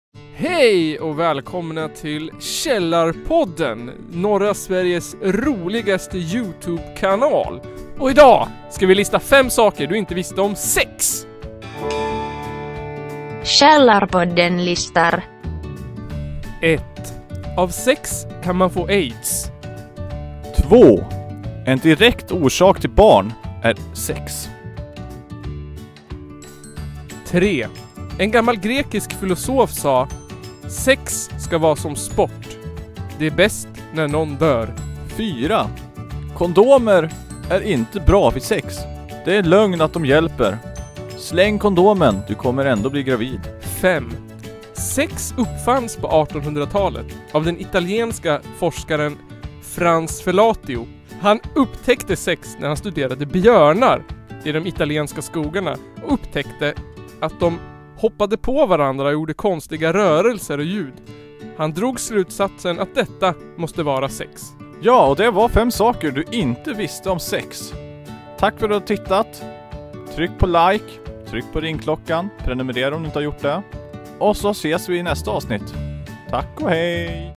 5 Saker du INTE visste om SEX!
Källarpodden listar 5 saker du inte visste om sex!

Vill du/ni kontakta oss med ett ämne att prata om eller en annan synpunkt så kan du höra av dig till oss på sociala medier eller på